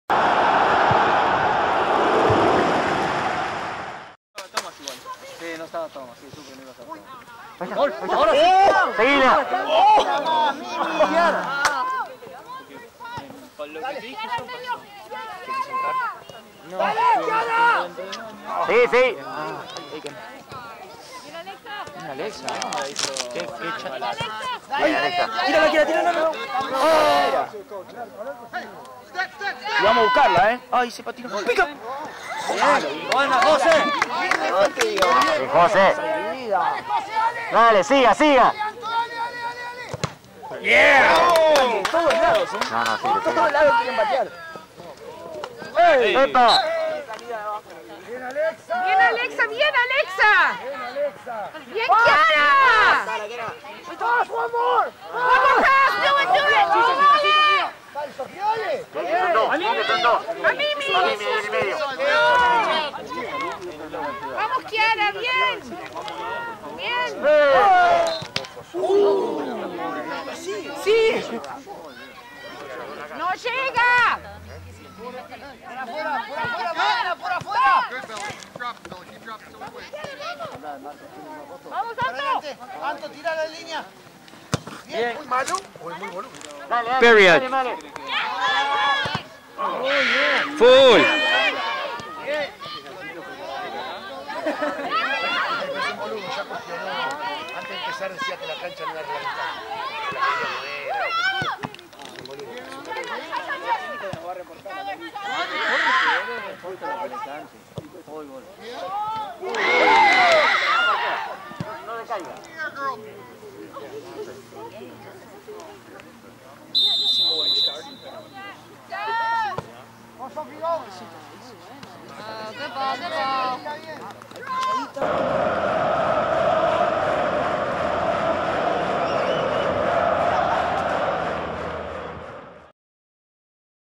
TOCATV: U16 Girls MYSTICS (some highlights from the game)
📈 EDPsoccer League
Sunday, October 25, 2020
U16 Girls MYSTICS vs A3 Manchester City